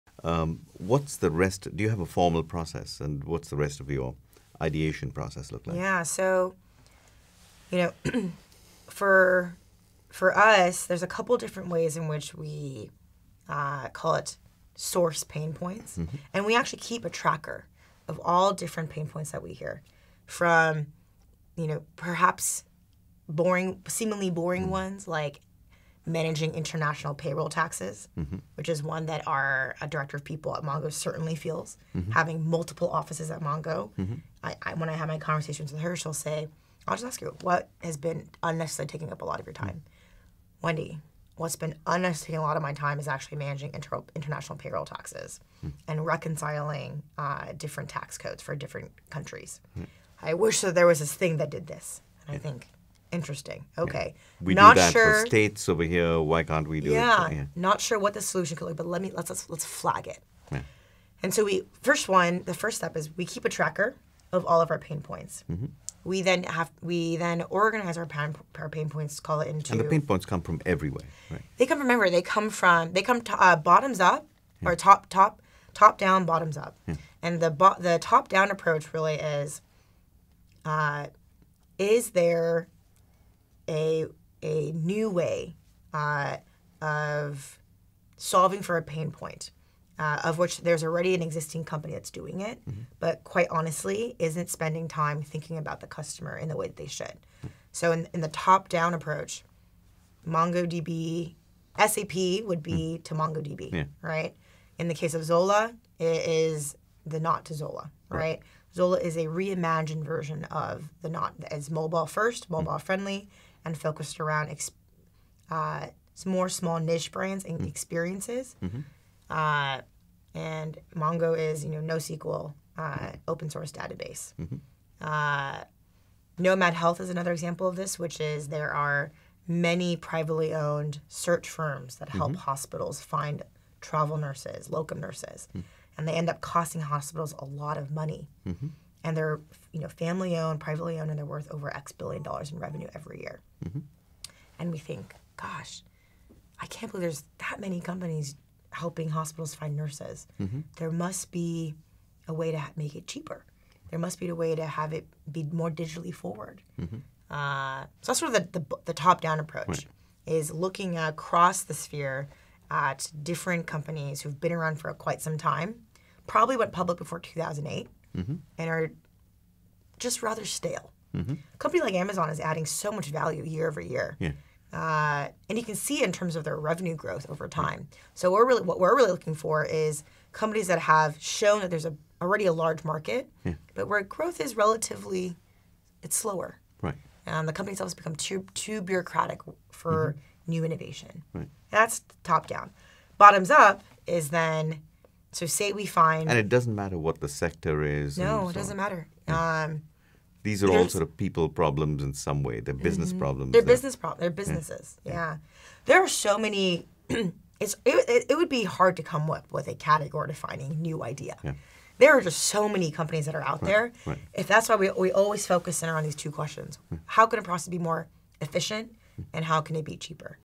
Diving into the Idea Process at AlleyCorp
Wendy Tsu, Partner of New Business Ventures at AlleyCorp, shares how AlleyCorp investigates and pursues new ideas.